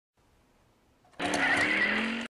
Toobalink - Build A Car Launch Ramp
This video shows how to construct a launch ramp for your Matchbox and Hot Wheels vehicles.